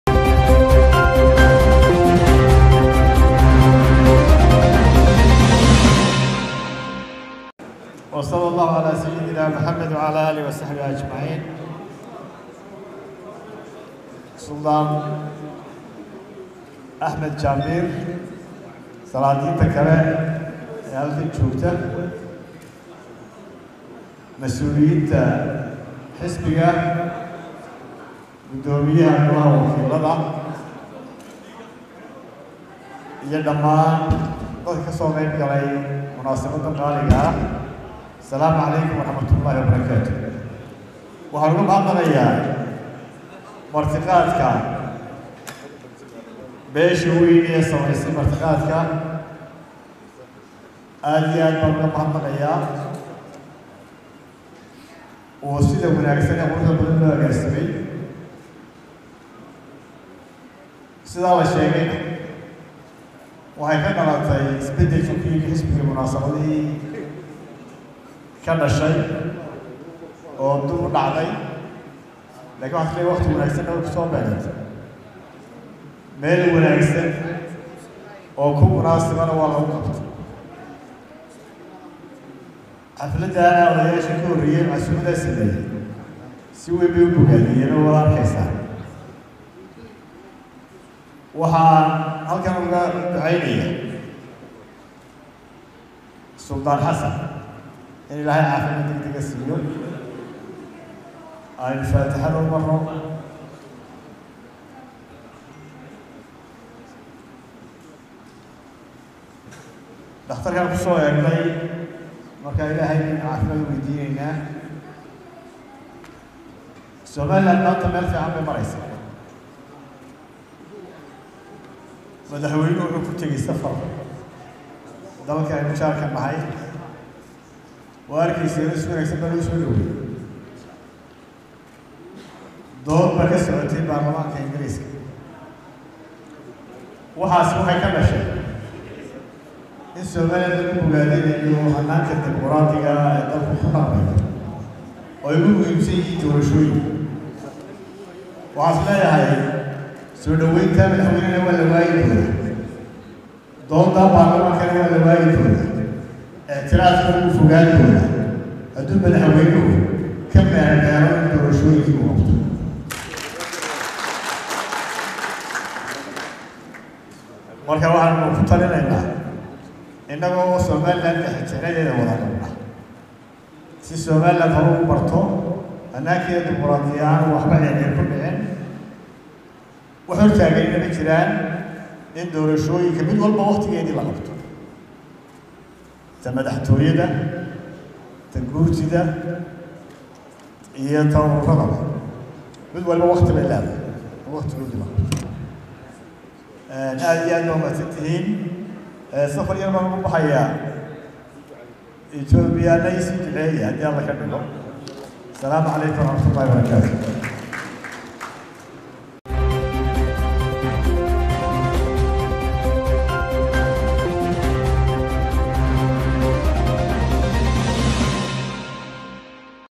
"Ictiraafkuna wuu Fogaan doonaa hadii Madaxweynuhu ka meer-meero Doorashoo..." Cabdiraxmaan Cirro.
Haddii Madaxweyuhu Doorashooyinka ka Meeraysto Helimaayo Ictiraafka Somaliland Musharax Cirro.